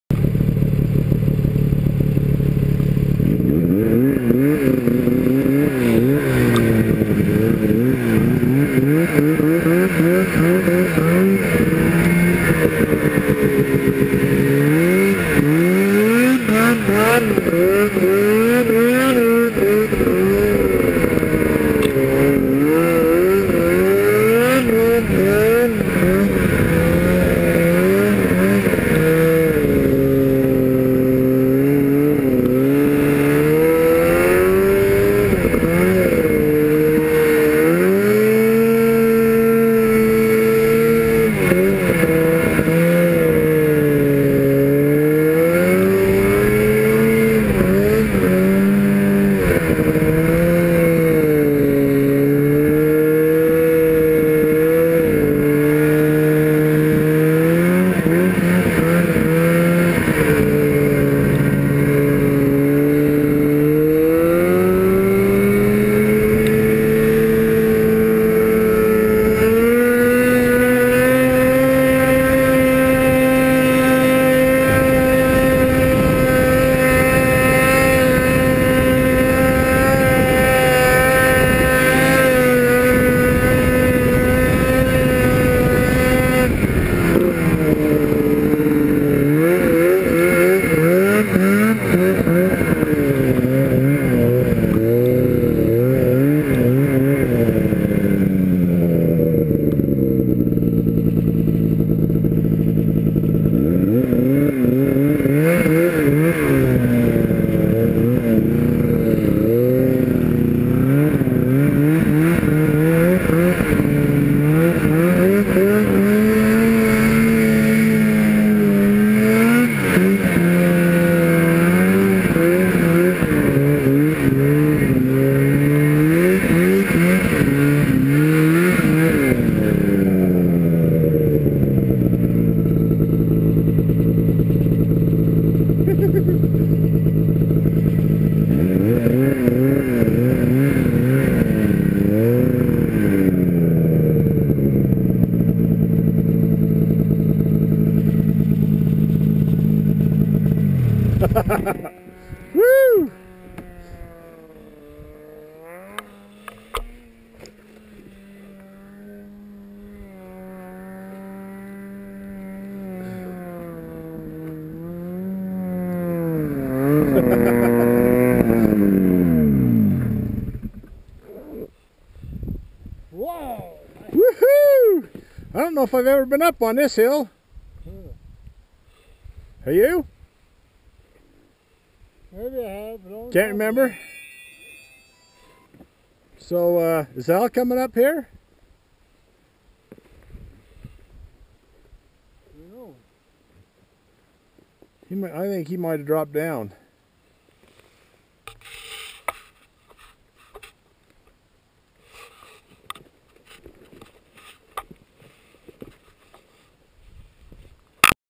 White Canyon Mar 27, 2014
Martin & Kyle at White Canyon Franklin Basin, top of the world.